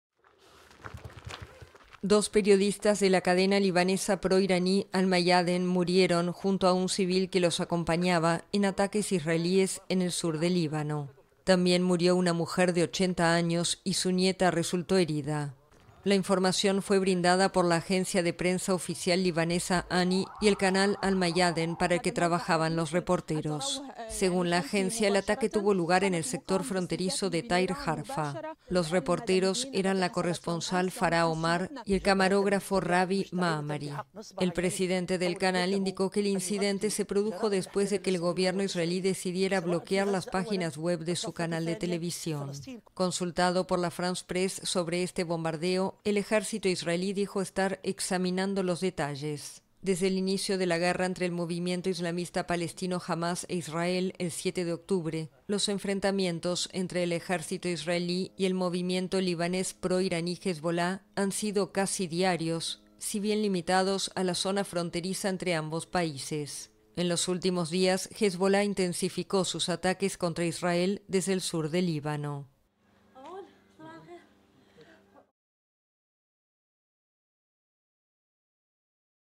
Mueren dos periodistas y otros dos civiles en bombardeos israelíes en Líbano
Cuatro civiles, entre ellos dos periodistas, murieron el martes en el sur del Líbano en ataques israelíes, indicaron la agencia de prensa oficial libanesa Ani y el canal Al Mayadeen, para el que trabajaban los reporteros.